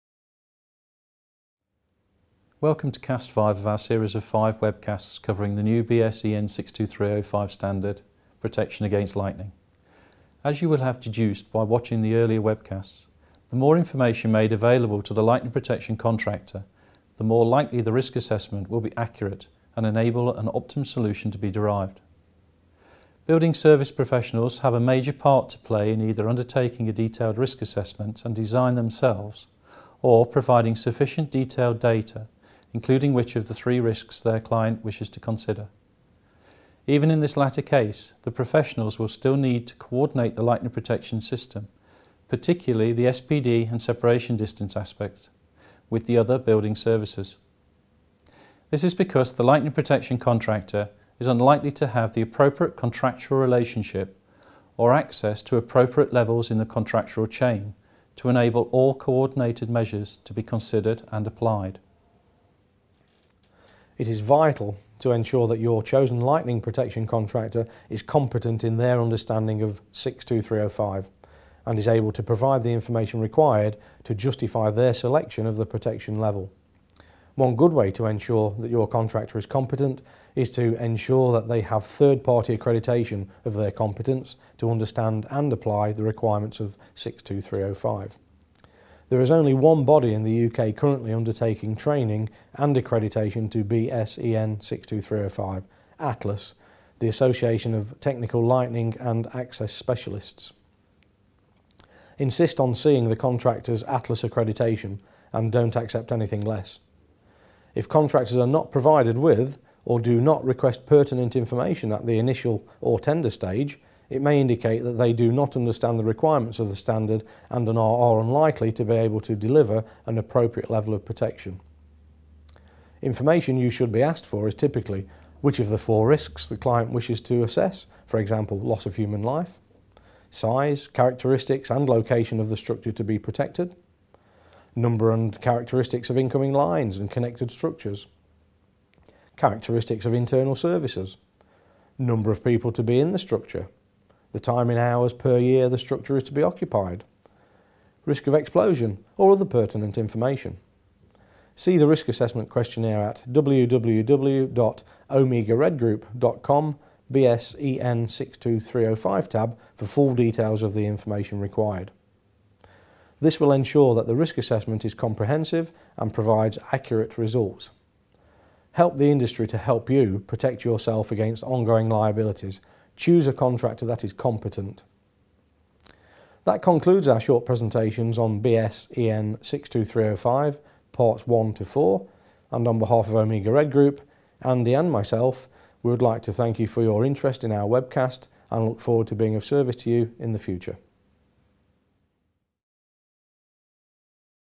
5 - BS EN 62305:2006 WEBCAST SUMMARY
Omega Red Group Ltd, the UK's leader in design and installation of Lightning Protection Systems, presents an in depth introduction to BS EN 62305 in 5 parts. All of these are available on our website in a larger player